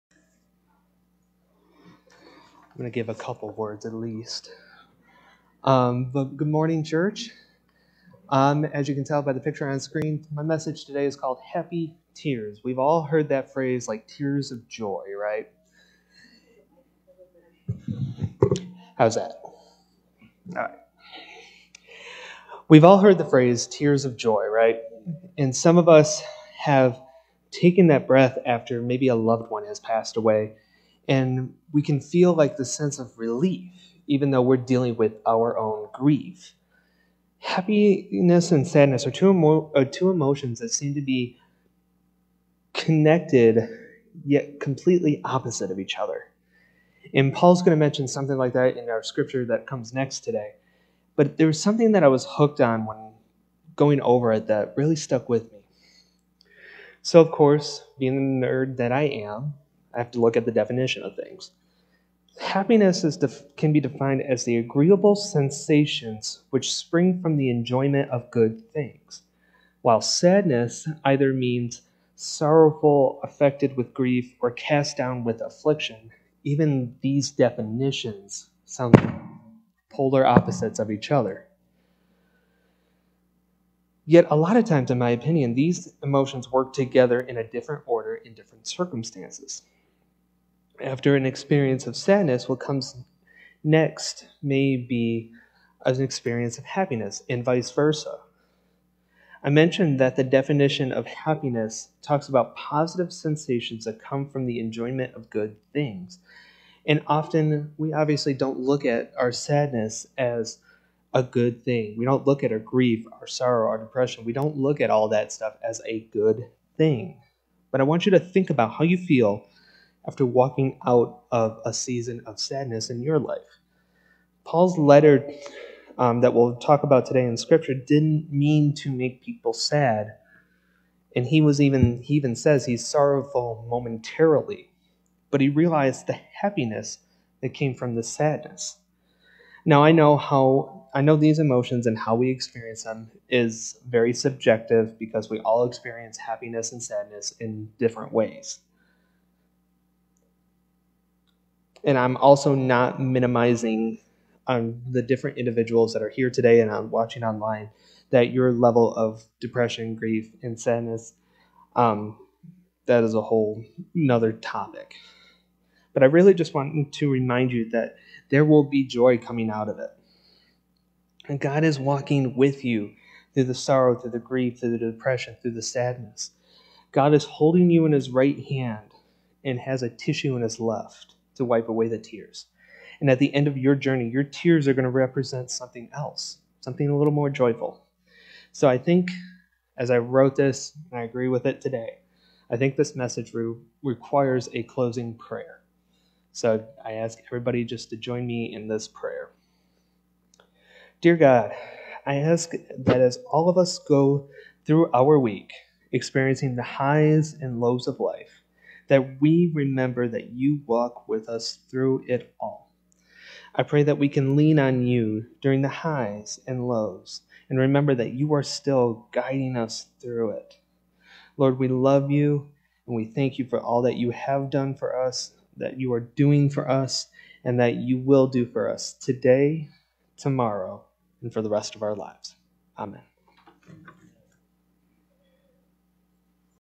Happy Tears || 08.01.2021
In this message, we learn about how our emotions can confuse us but God gives them to us for a reason!